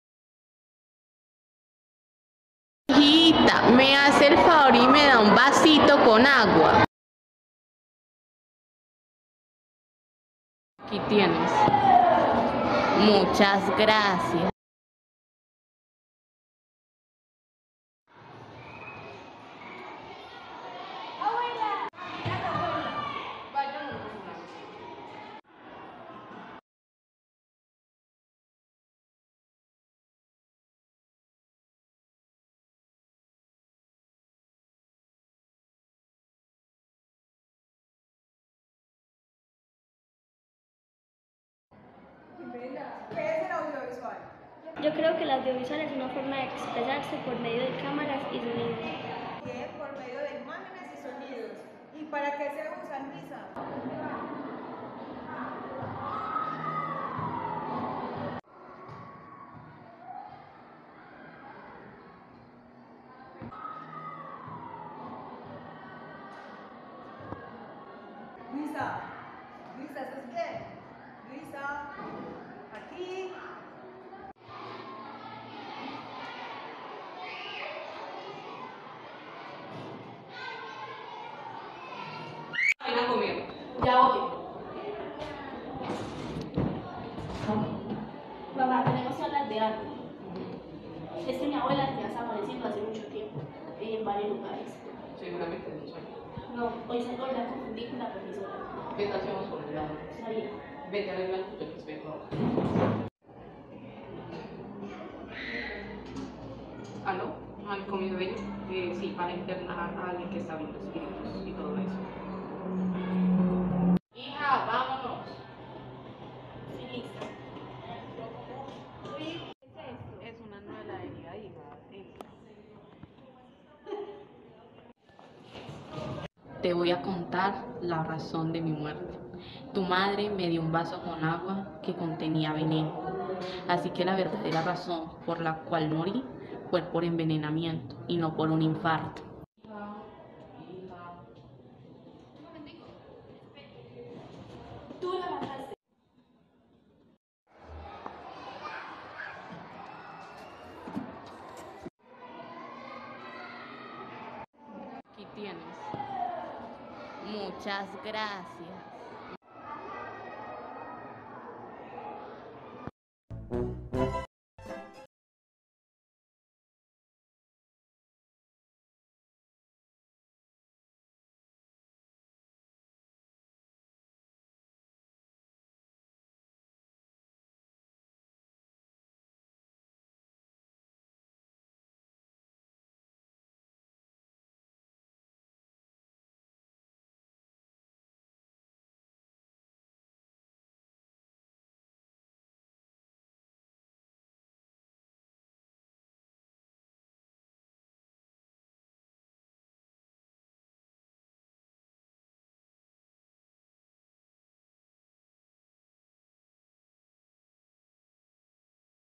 La abuela fantasma
Una jóven ve el fantasma de su abuela y descubre el secreto tras su inesperada muerte.